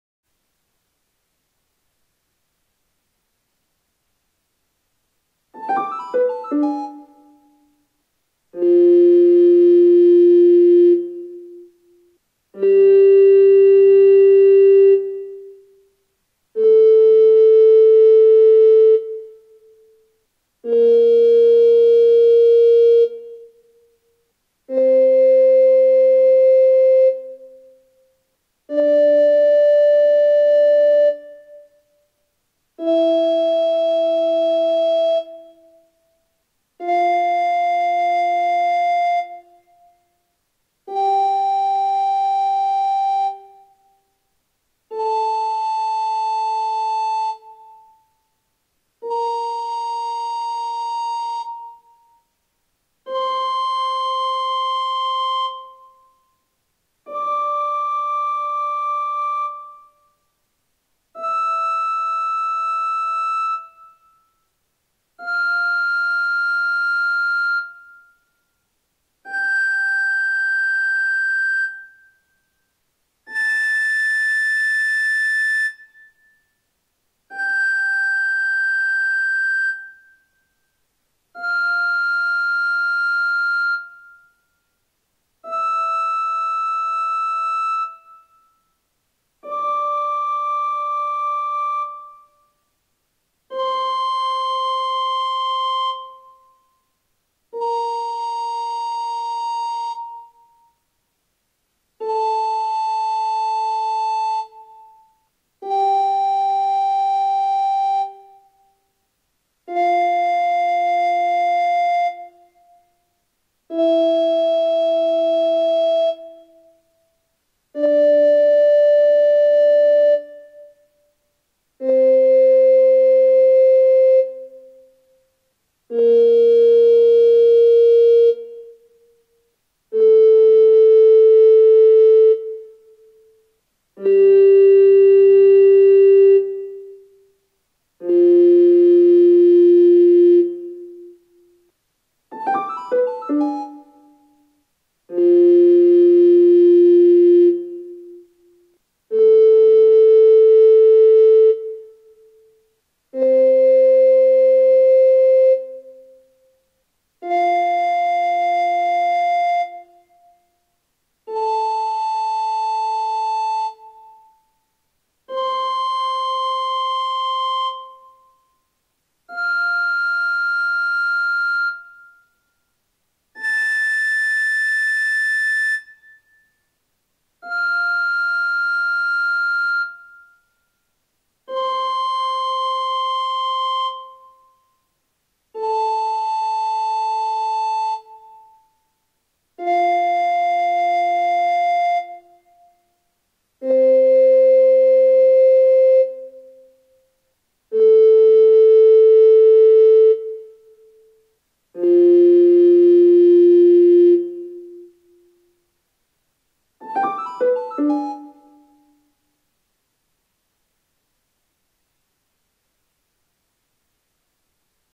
Escala e arpejo - fá maior
Scales and arpeggios - f major - treble recorder
For didactic use

Escala e arpejo - fá maior - flauta de bisel contralto
Para fins didáticos